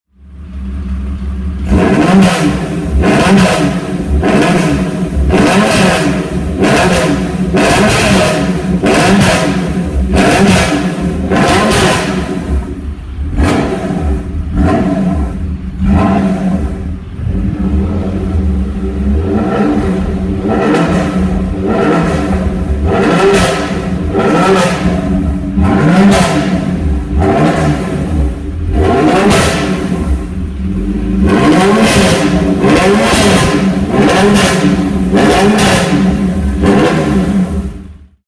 TVR Sagaris GT Project Car - AES Exhaust System Pt. 3
A small video of the AES system. Much still needs to be done to connect the system up.